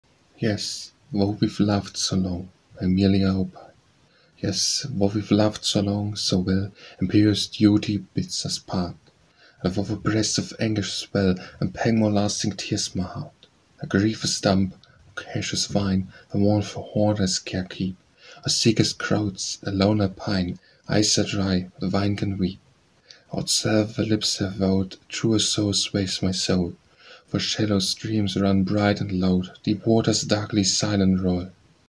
Yes Though We ve Loved so Long - Amelia Opie
Yes Though We ve Loved so Long
Amelia Opie

Yes .though we've loved so long, so well,
Imperious duty bids us part;
But though thy breast with anguish swell,
A pang more lasting tears my heart.
My grief is dumb,. loquacious thine,
The mournful hoard I sacred keep;
Thou seekest crowds, alone I pine;
My eyes are dry, but thine can weep.
Then, whatsoe'er thy lips have vowed,
A truer sorrow sways my soul;
For shallow streams run bright and loud,
Deep waters darkly silent roll.